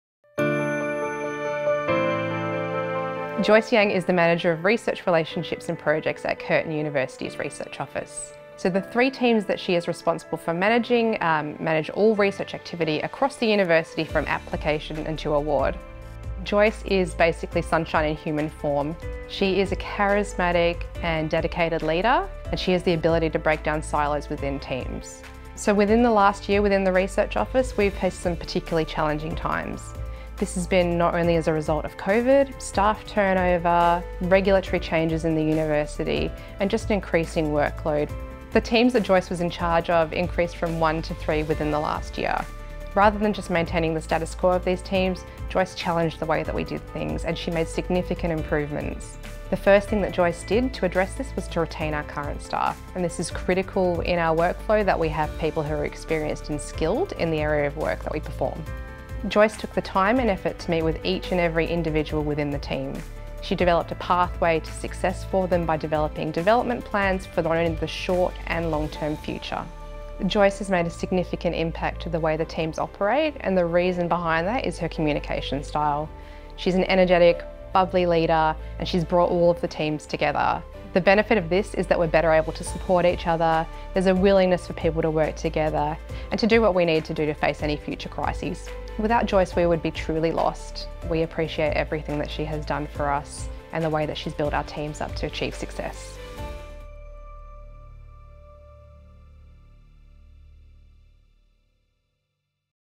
Excellence in Values-based Leadership – Joyce Yang
The development of a more inclusive, supportive, and adaptable workplace saw Joyce demonstrate Integrity and Courage in her values-based leadership and challenges she took on.

Joyce’s leadership style has always put staff wellbeing front and centre, investing considerable time with her team to provide advice, mentorship and transparent communication.

Joyce’s contribution to a sustainable future for the University has resulted in the University to continue to operate successfully with minimal impact to its clients; morale to be maintained in very challenging times and introduction of new methods of leadership and team workings.

The VC’s Excellence Awards for Professional Staff recognise and celebrate the vital contributions in collaboration, innovation, customer service, values-based leadership, reconciliation and health and safety that our professional staff make to Curtin.

Congratulations to the winners who were announced at an awards presentation ceremony on 23rd of November 2022.